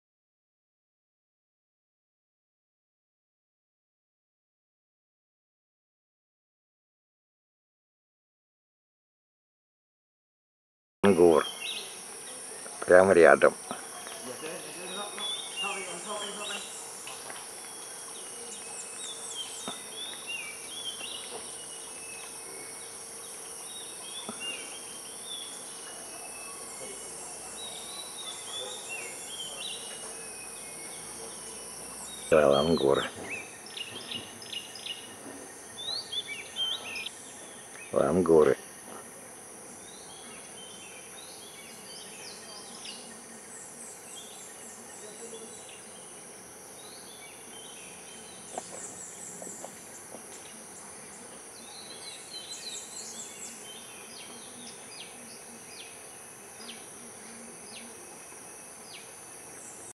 Oчковый лангур (Presbytis obscura)
Снято на территории научно-исследовательской станции заповедника Хала Бала, сектор Бала, Южный Таиланд.